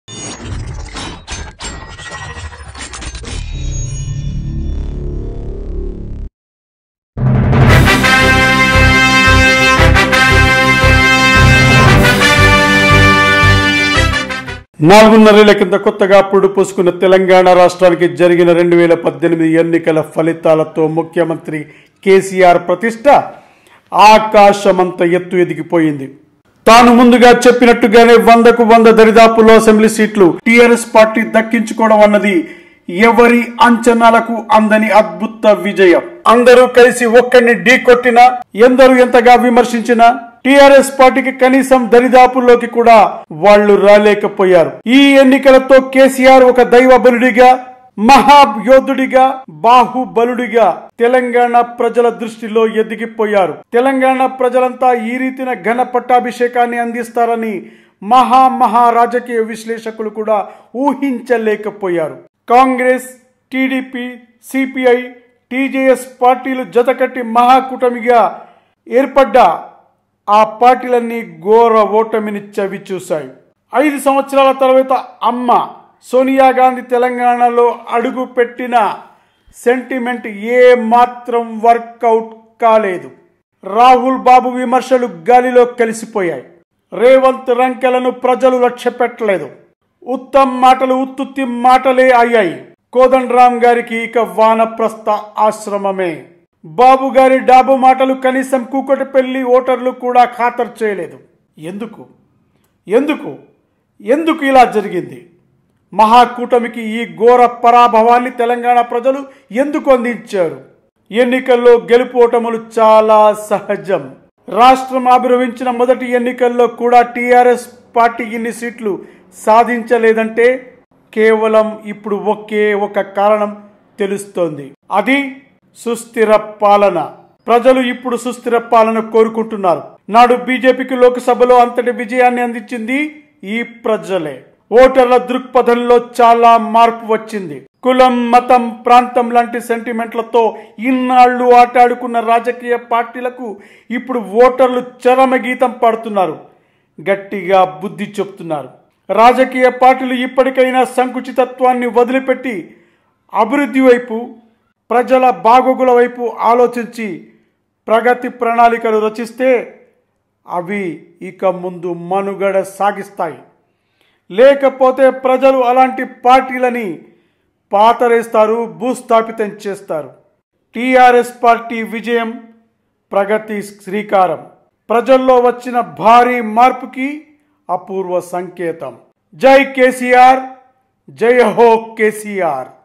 కారు హోరు|TRS CREATED NEW HISTORY|ఫిర్ కె సి ఆర్|మహాకూటమి కి ఘొరపరాభవం
Telangana Election Results 2018| TRS Ahead in over 80 Seats